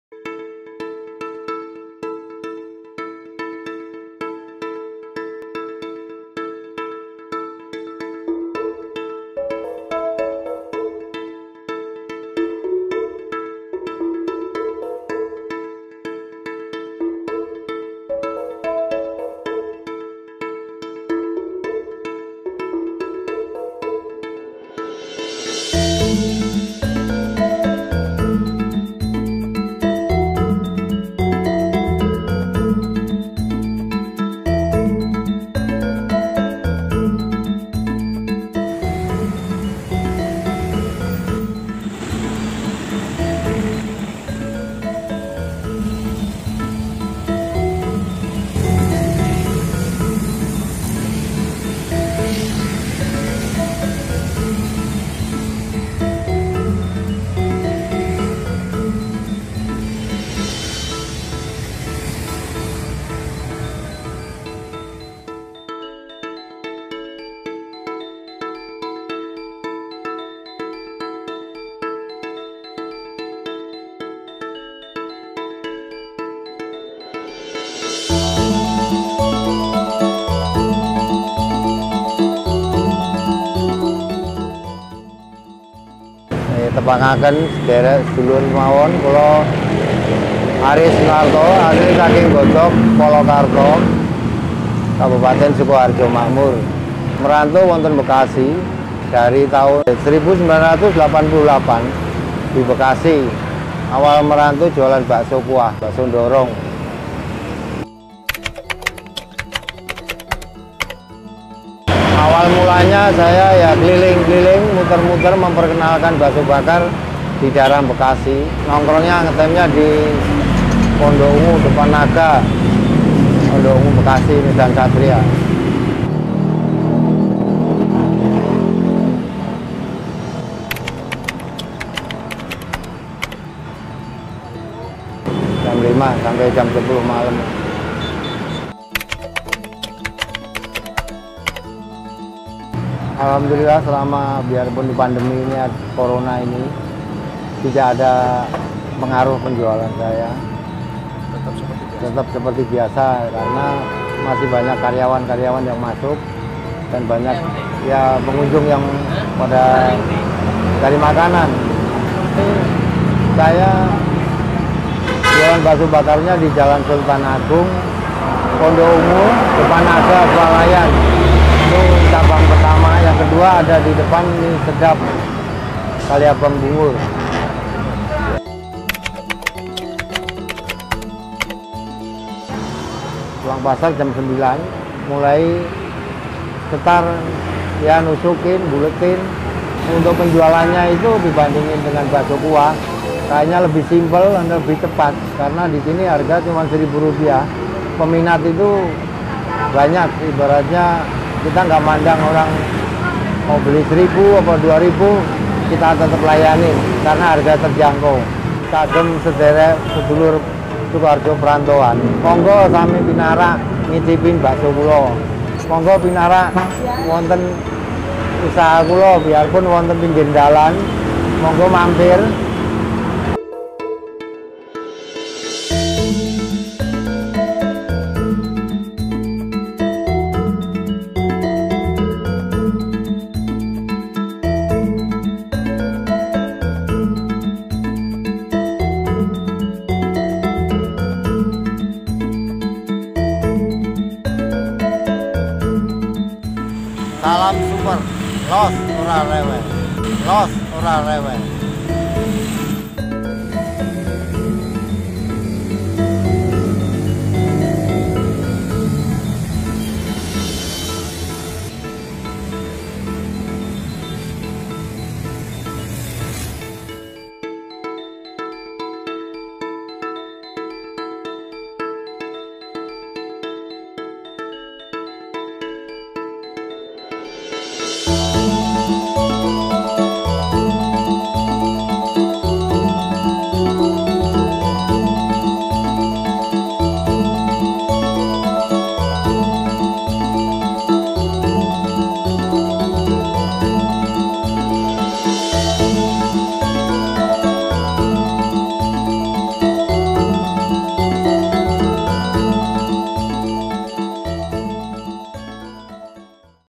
Bakso Bakar Pak Aris - Pondok Ungu | Murah Meriah Rp. 1000/tusuk- Dijamin Ketagihan
Aris Sunarto( 55th) asli Dari Desa Godog Kecamatan Polokarto Sukoharjo.

Merantau di Kota Bekasi sejak tahun 1988, awal mulanya berjualan bakso keliling.
Dari tahun 2013 mencoba beralih ke Bakso Bakar.
Pembeli selalu mengantri di jam jam tertentu, yaitu jam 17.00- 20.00, berangkat dari rumah jam 16.30 dan dagangan selalu habis di bawah jam 21.00(jika cuaca cerah)

Bakso Bakar Pak Aris, berjualan mangkal menggunakan sepeda motor di pertigaan Aqua/Depan Naga Swalayan Pondok Ungu Medan Satria Kota Bekasi.
Bakso Bakar Pak Aris,dibanderol 1000rp/tusuk..
MURAHNYA KEBANGETAN KAN..
RASANYA DIJAMIN KETAGIHAN..

Yuk.. Cobain..